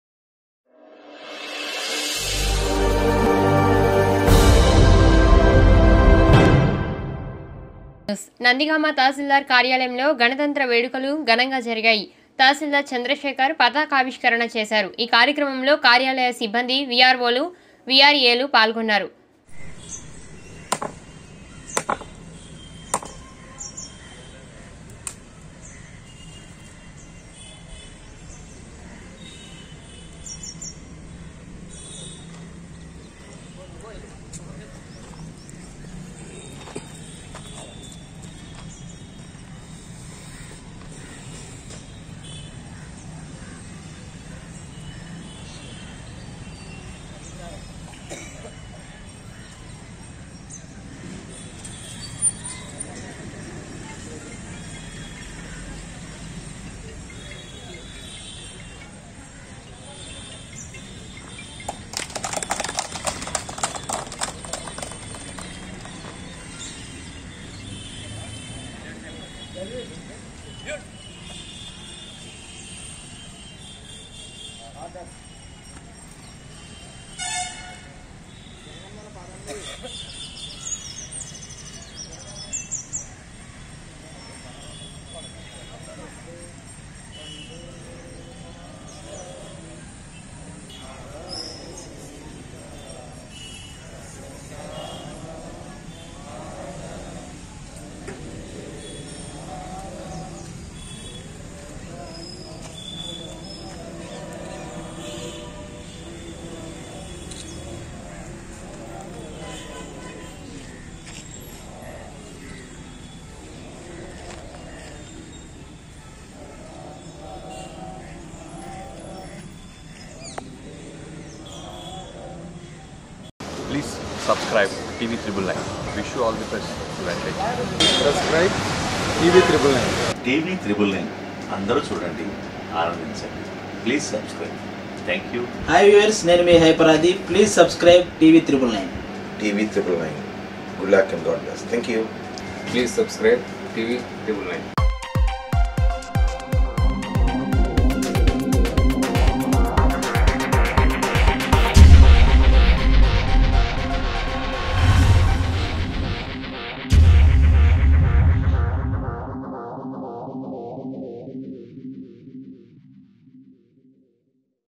నందిగామ తాసిల్దార్ కార్యాలయంలో ఘన'తంత్ర వేడుకలు ఘనంగా జరిగాయి తాసిల్దారు చంద్రశేఖర్పతాకావిష్కరణ
నందిగామ తాసిల్దార్ కార్యాలయంలో ఘన'తంత్ర వేడుకలు ఘనంగా జరిగాయి తాసిల్దారు చంద్రశేఖర్ పతాకావిష్కరణ చేశారు ఈ కార్యక్రమంలో కార్యాలయ సిబ్బంది వి ఆర్ వో లు వీఆర్ఏలు పాల్గొన్నారు